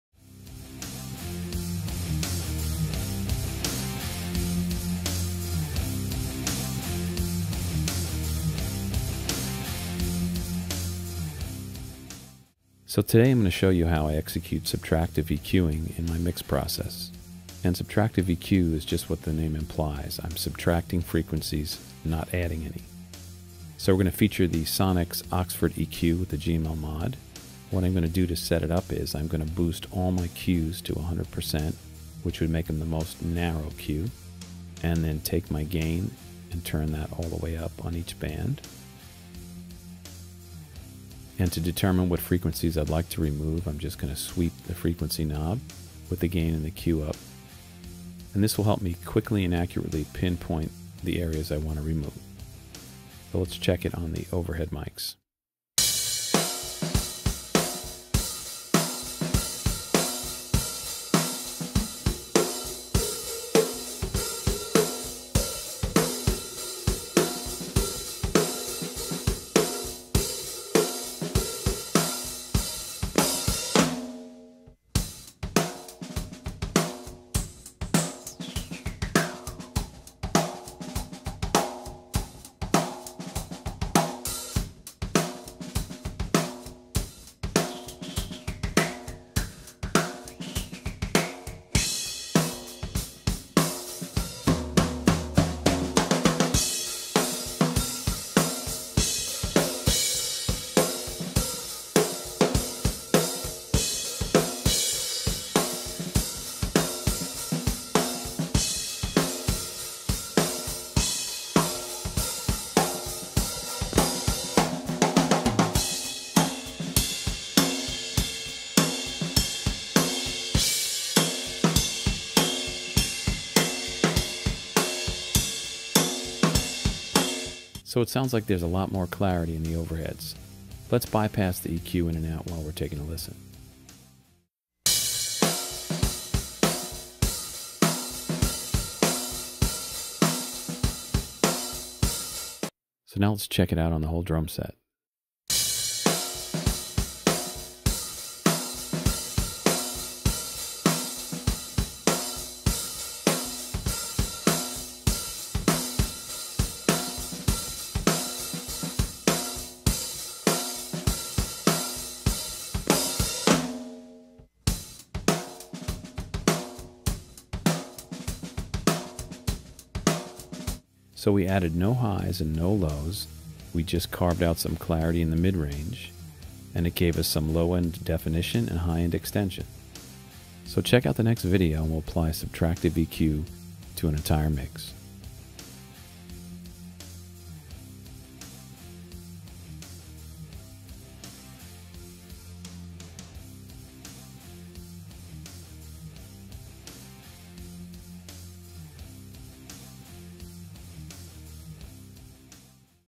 Pro Tools Online Virtual Mixing-How to use subtractive EQ in your mixes - Part 1 - Drums
How to use subtractive EQ in your mixes - Part 1 - Drums